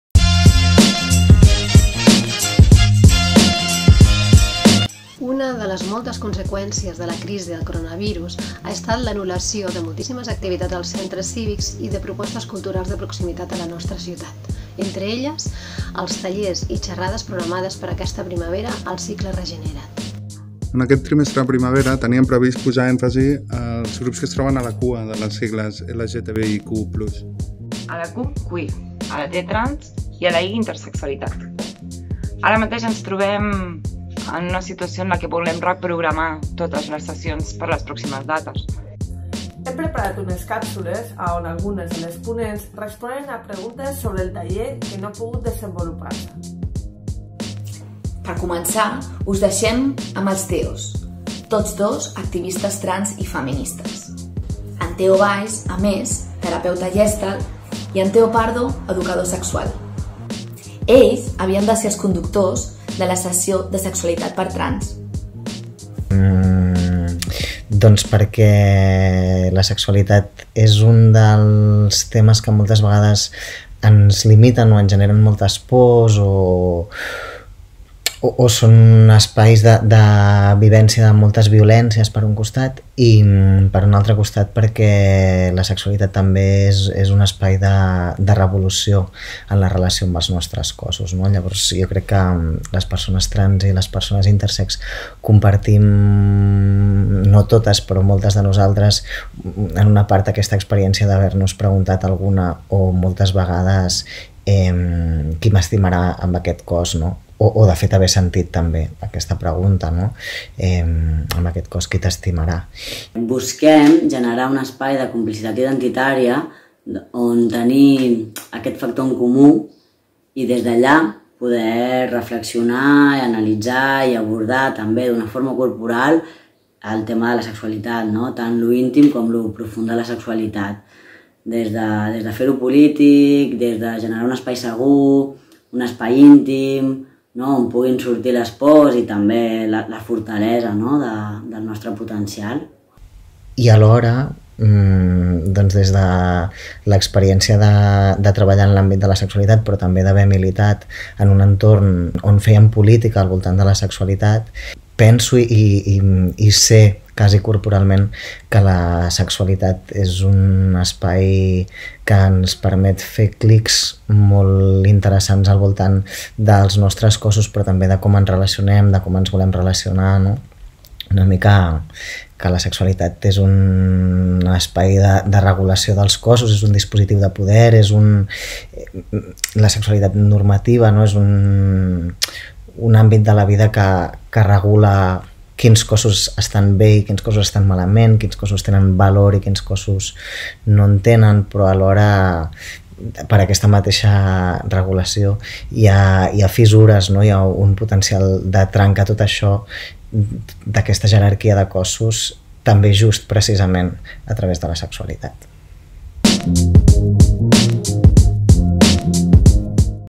ReGenera't amb els Teos - [LGTBIQ+ Confinat] 1a pregunta_trans
1a pregunta: Per què un taller de sexualitat per a persones trans?